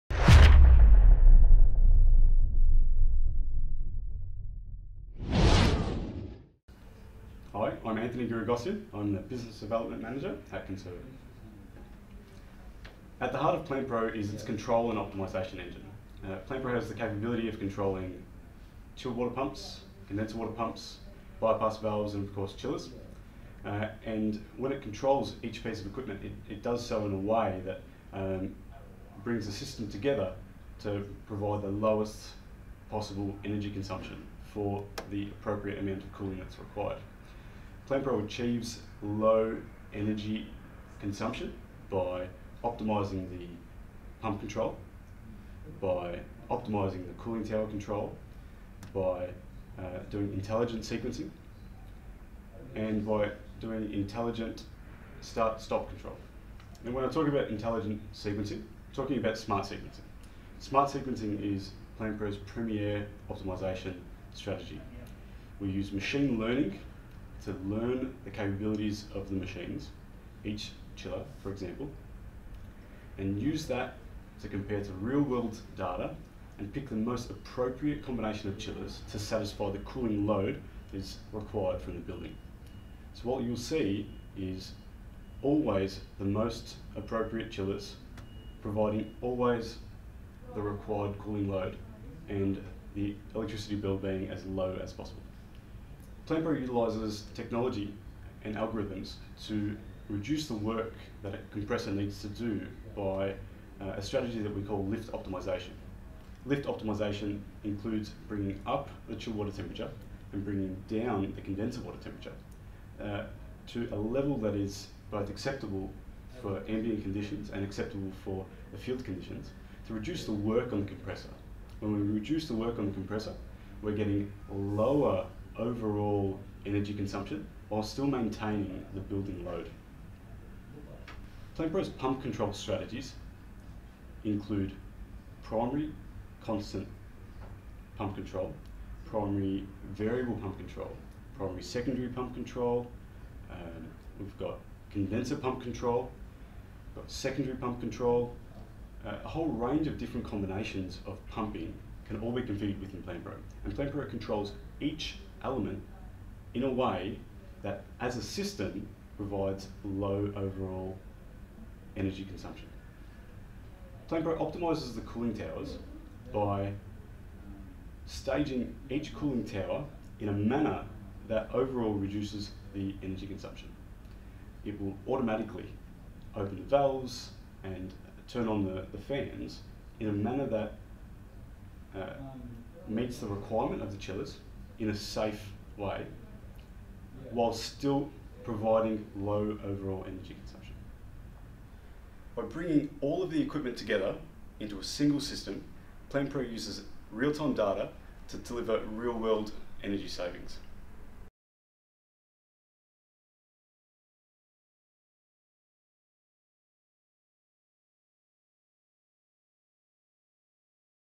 PlantPRO 2.0 – Central plant control and optimisation
Conserve It’s Business Development Manager, Anthony Guiragossian, discusses the active intelligent control and optimisation strategies that give PlantPRO the ability to save vast amounts of electrical energy for end customers. PlantPRO is an acronym for Plantroom Performance Reliability and Optimisation and it does not compromise performance and reliability of central plant operation to provide optimisation.